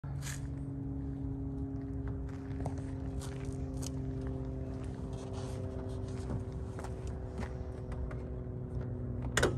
Will I fit in a Bugeye Sprite?
Many of our prospective clients ask us, "Will I fit in a Bugeye Sprite?" Well, 6'3" Stephen is here to say... yes! Even with a hardtop installed, vertically gifted British car lovers can still get into a Bugeye!